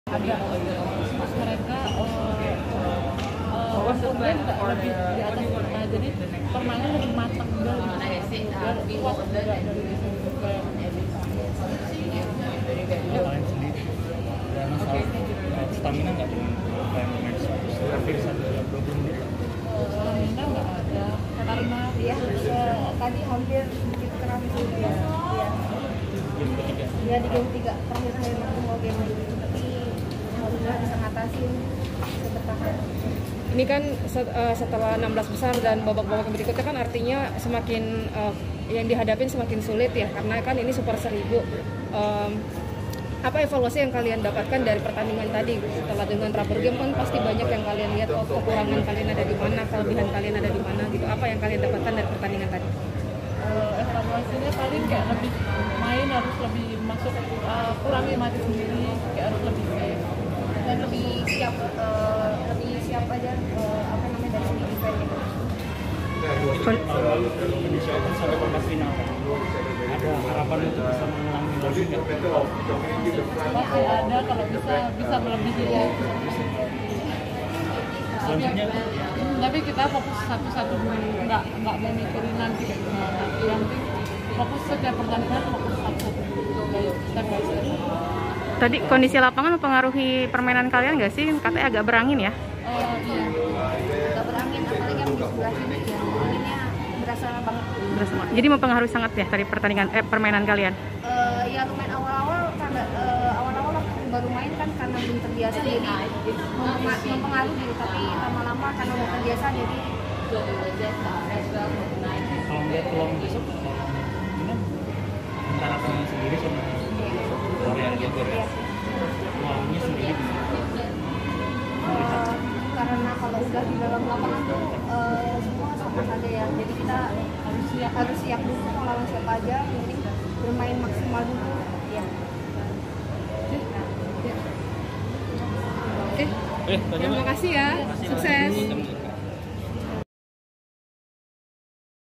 Febriana/Amalia Ceritakan Keberhasilan Lolos ke Babak Kedua Indonesia Open 2022
Indonesia Open Super 1000 tahun 2022